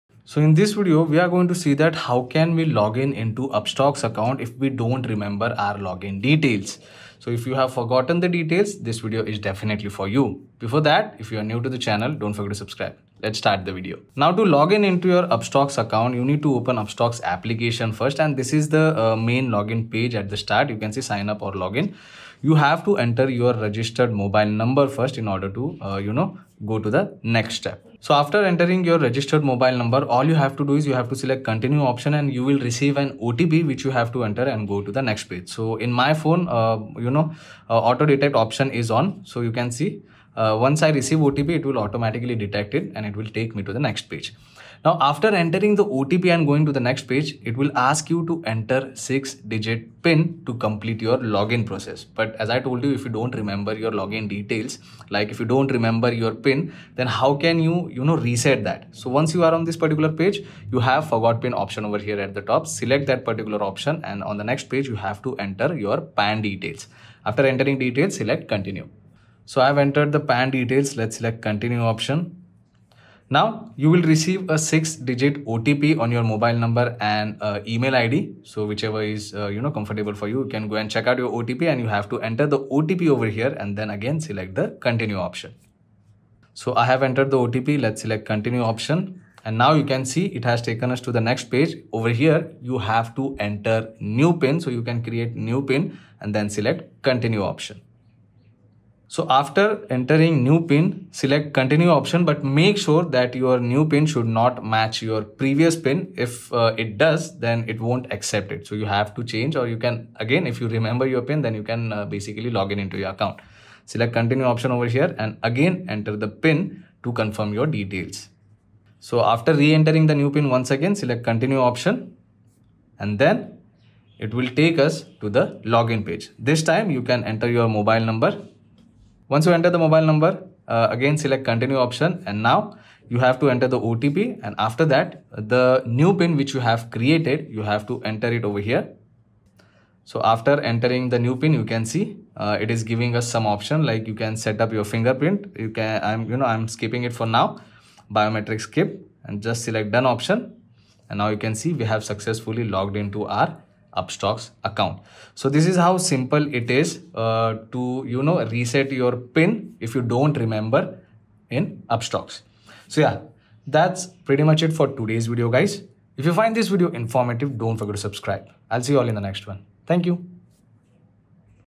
Upstox Forgot PIN - How to Change or Reset PIN in Upstox Application?
Understand how to reset your PIN in Upstox in case forgotten. This video in English helps you understand how you can change or reset your PIN in the Upstox application. To change your PIN in the Upstox application open the Upstox app on your mobile, login to your account, and follow the steps given in the video.